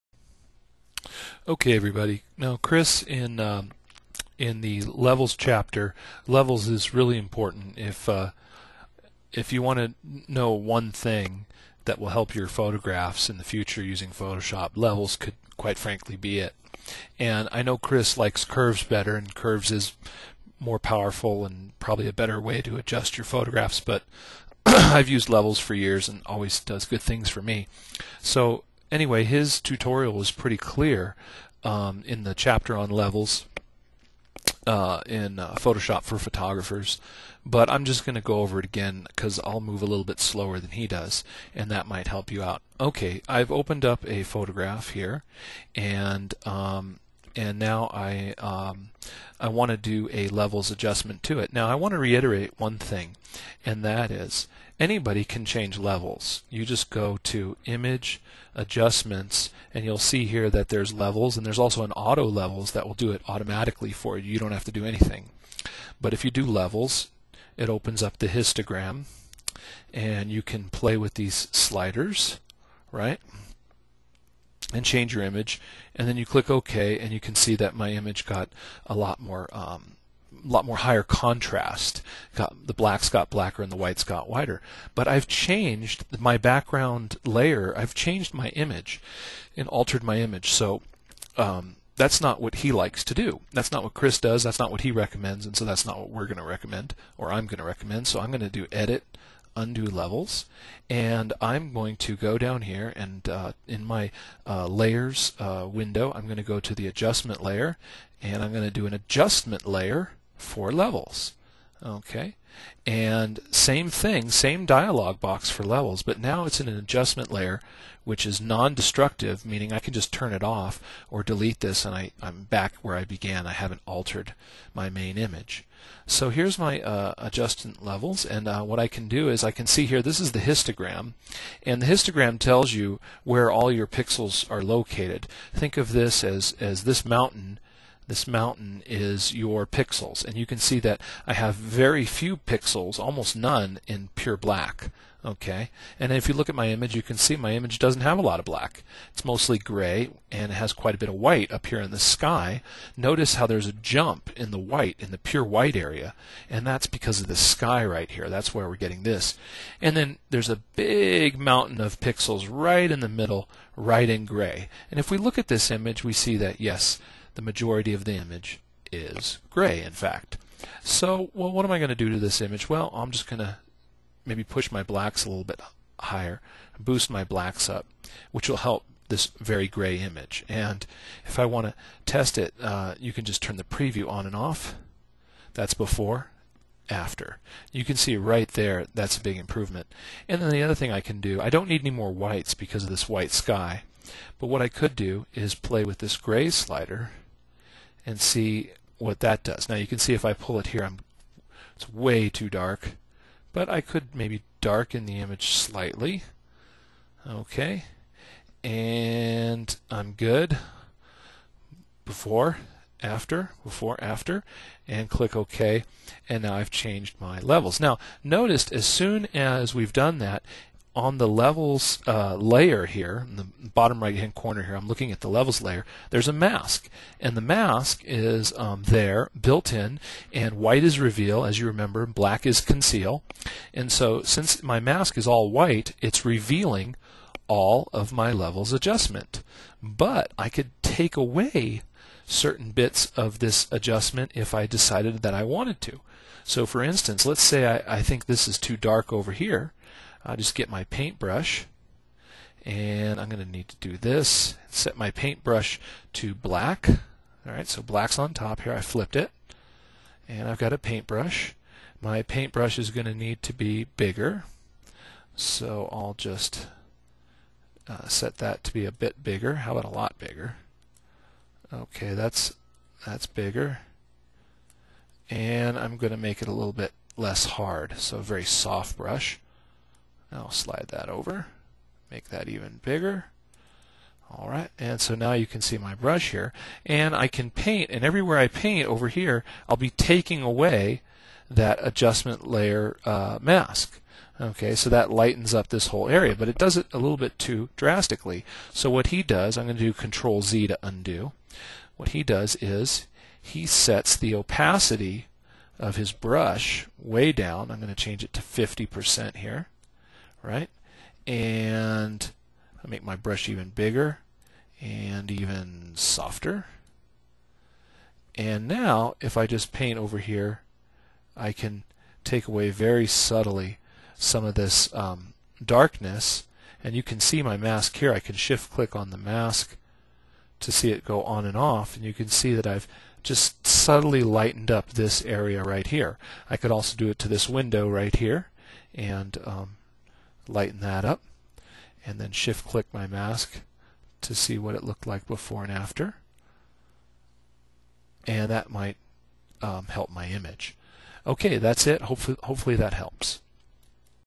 photoshop levels
A brief tutorial on working with levels adjustment layers in Photoshop CS3